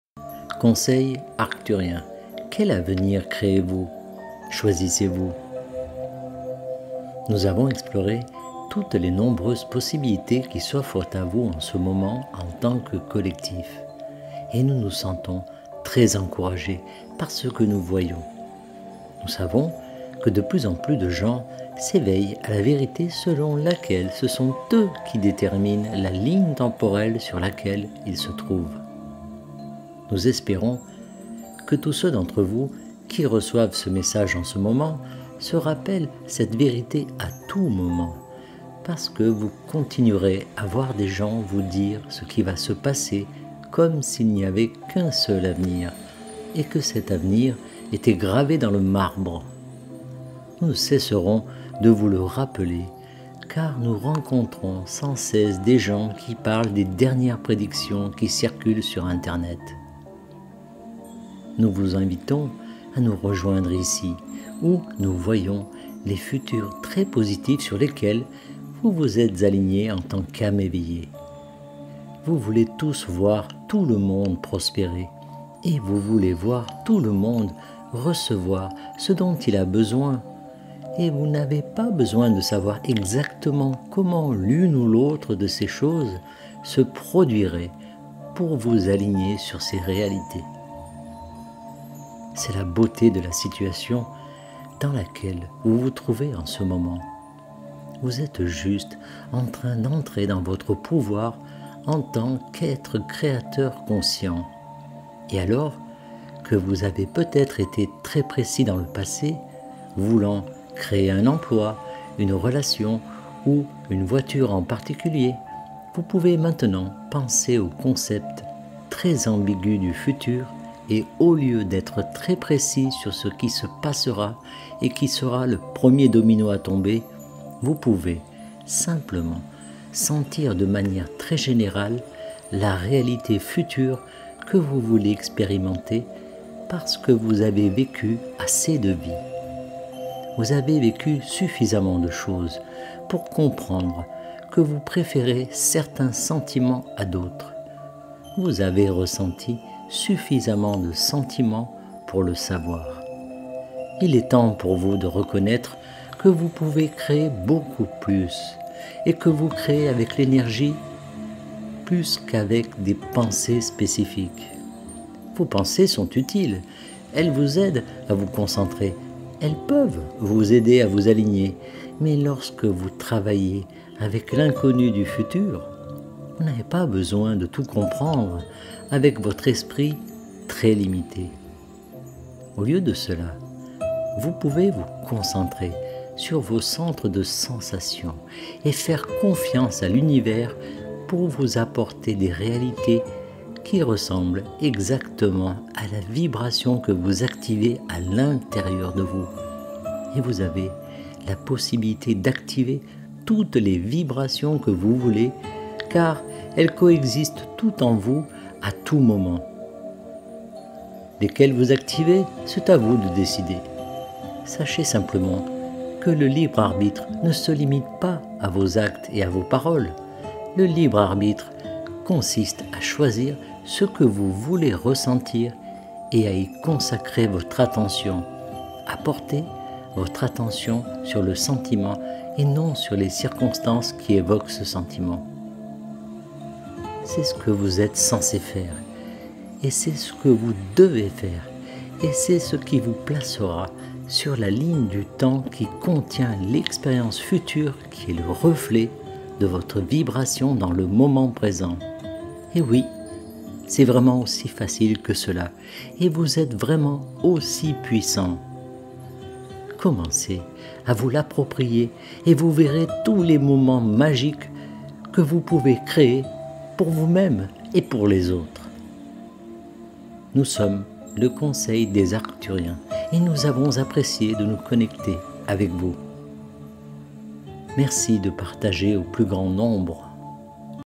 Conseil Arcturien : Quel avenir créez-vous/choisissez-vous ?
Nous avons exploré toutes les nombreuses possibilités qui s’offrent à vous en ce moment, en tant que collectif, et nous nous sentons très encouragés par ce que nous voyons. Nous savons que de plus en plus de gens s’éveillent à la vérité selon laquelle ce sont eux qui déterminent la ligne temporelle sur laquelle ils se trouvent.